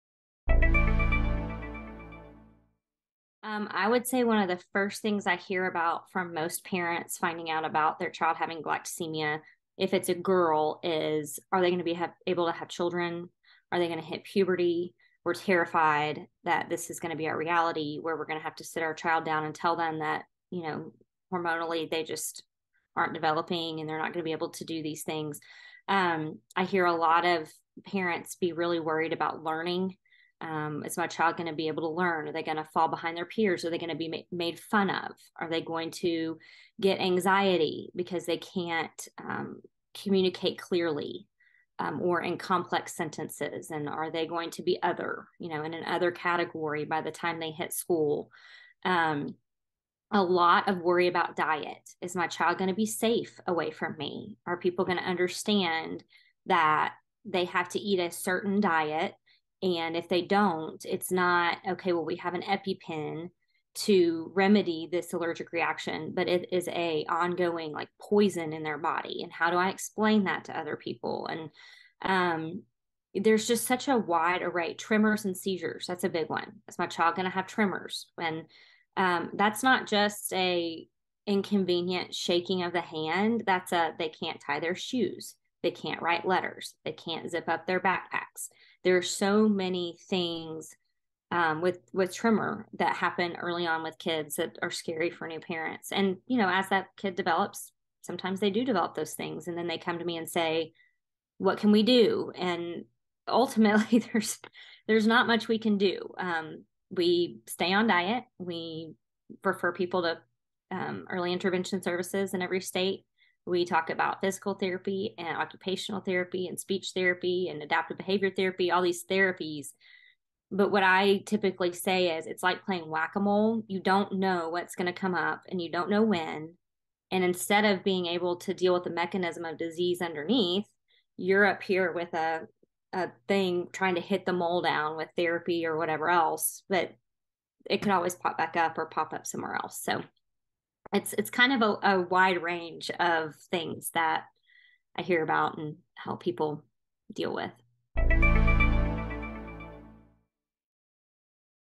Common Concerns Parents Have about Galactosemia
Brittany Cudzilo, Vice President of the Galactosemia Foundation, and parent of two children with galactosemia, discusses some of the common questions parents have about this rare condition.

Galactosemia is a metabolic disorder that affects a child's ability to metabolize galactose, which is present in many foods, including dairy products. It is inherited in an autosomal recessive manner.

Symptoms of galactosemia may include failure to thrive, gastrointestinal problems, hypoglycemia, and liver dysfunction. If left untreated, galactosemia can lead to serious complications such as cataracts, intellectual disability, speech difficulties, and neurological problems.

Strict adherence to a galactose-restricted diet can help manage the symptoms and reduce the risk of complications. However, even with dietary management, many children with galactosemia may still experience intellectual disabilities and other problems.

As noted by Brittany, parents of children with galactosemia have many concerns, including how their child will develop and interact with others.